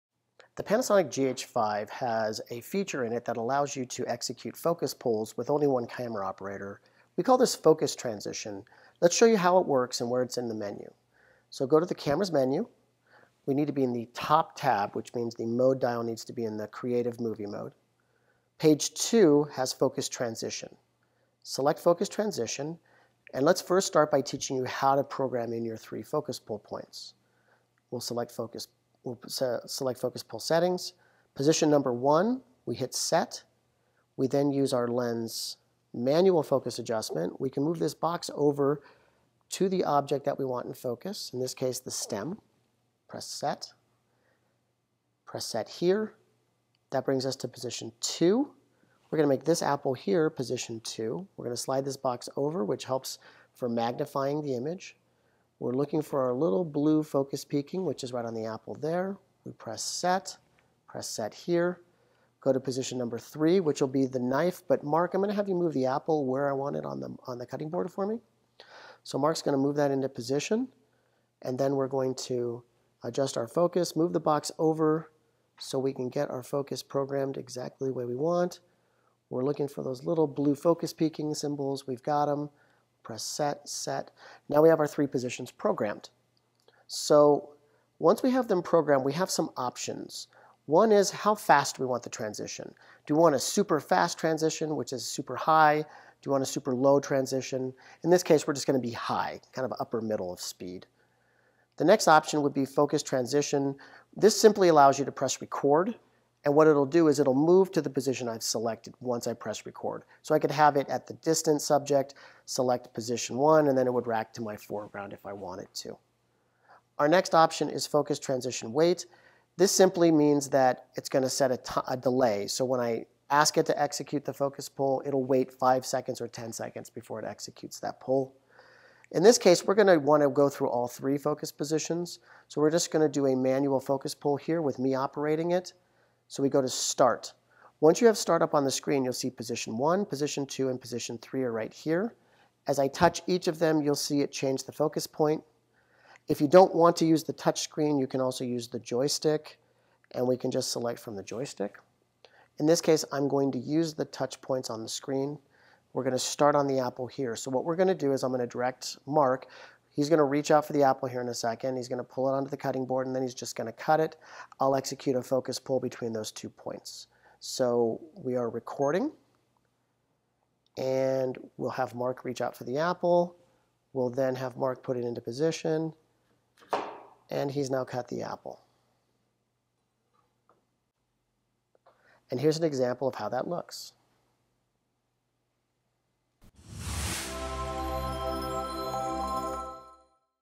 GH5 Focus Transition Function
Panasonic's Tech Team provides you with a quick explanation about how to set up your new GH5!